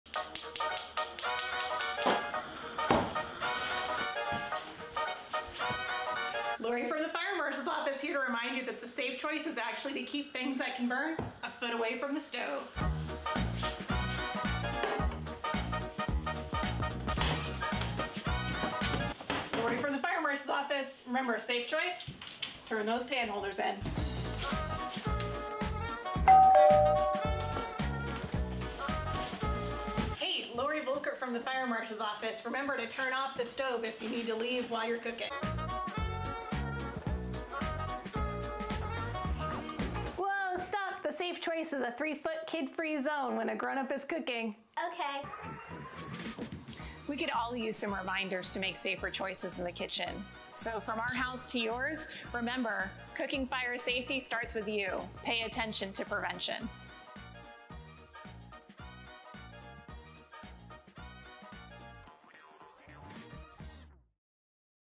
Kitchen Fire Safety
We can all use a reminder to make safer choices in the kitchen. Here are a few tips to help you prevent the most common causes of kitchen fires and burns.